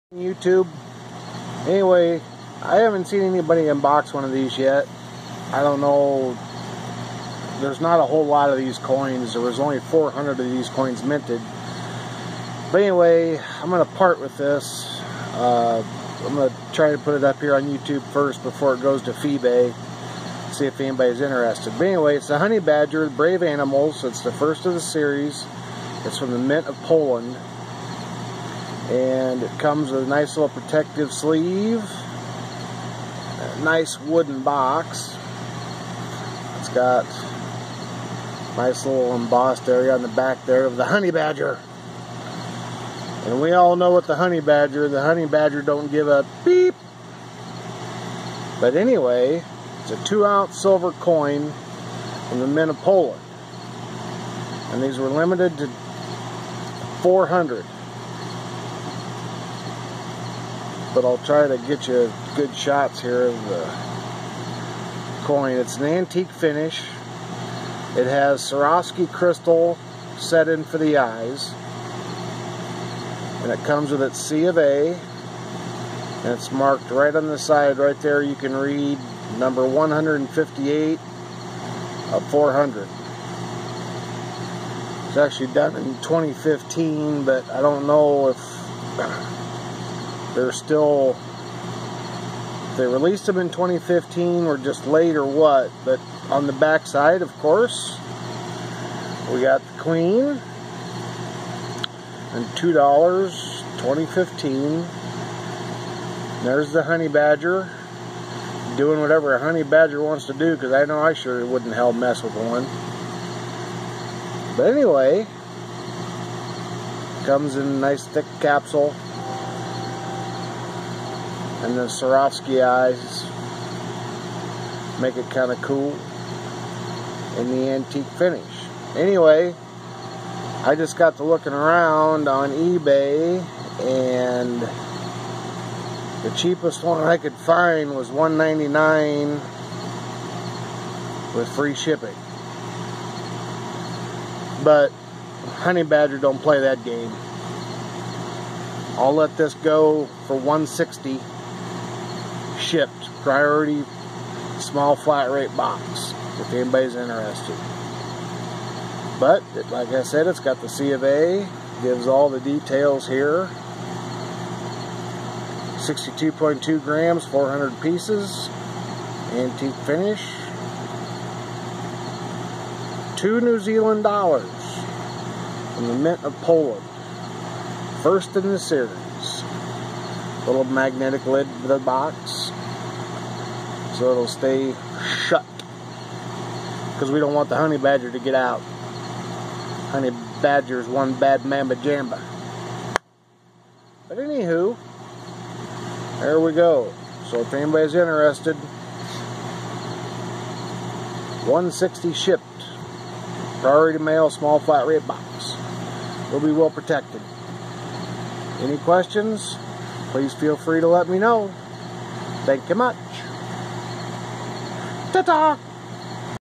4 sale. Honey Badger 1st in series. 158 of 400. I'll sell for 160.00 shipped psflb
Mint of Poland. 2 ounces of .999 silver Swarovski crystal set eyes. Beautiful coin. Any questions let me know.